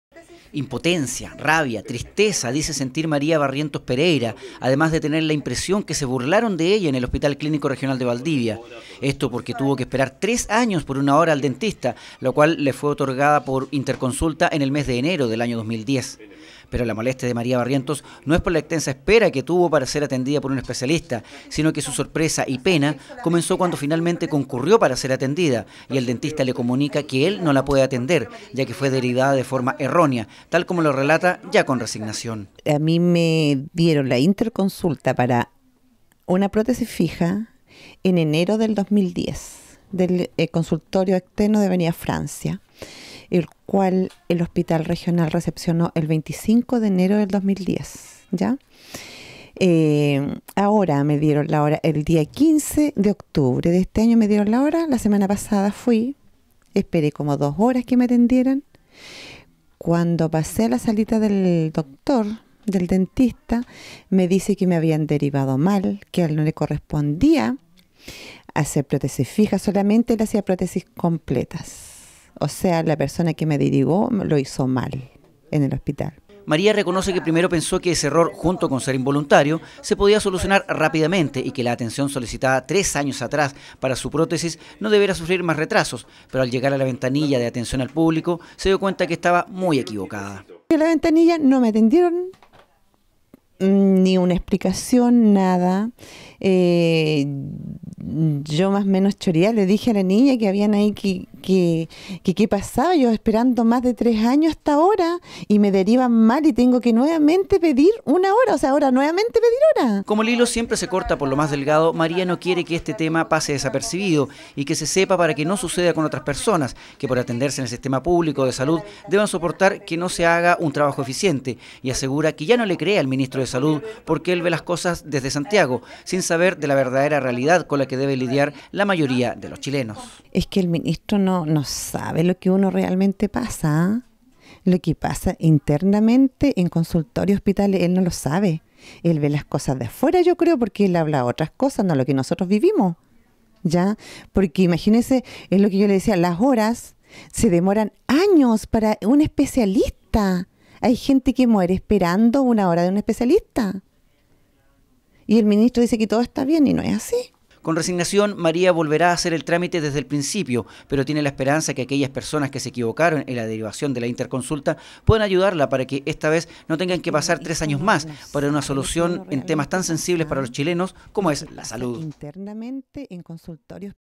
Mujer esperó 3 años por atención en hospital de Valdivia
En el Hospital Clínico Regional de Valdivia, una mujer espero tres años por atención que lamentablemente, por presunto error de funcionaria,  no se ejecutó.